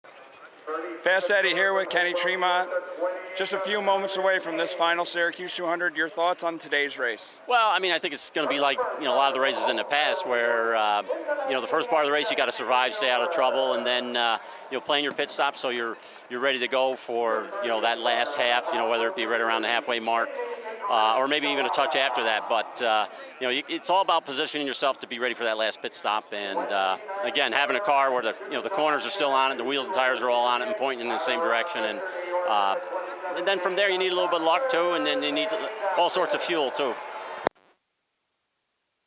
Super Dirt Week 2015 - Kenny Tremont Sunday Pre-race interview
Kenny Tremont starts 9th